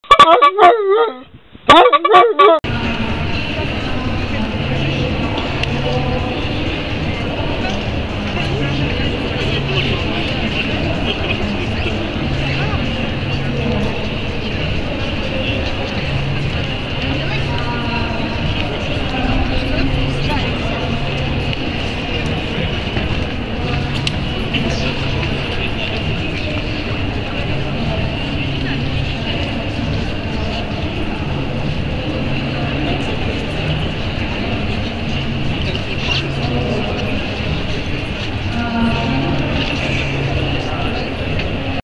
Биороботы из Японии. Киборги реалистично наступают на пятки секс игрушкам
28 марта, 2013 - Неделя моды в Москве. Наступление киборгов нового поколения. Биороботы из Японии. Киборги реалистично наступают на пятки секс игрушкам
Записки деревенского модника#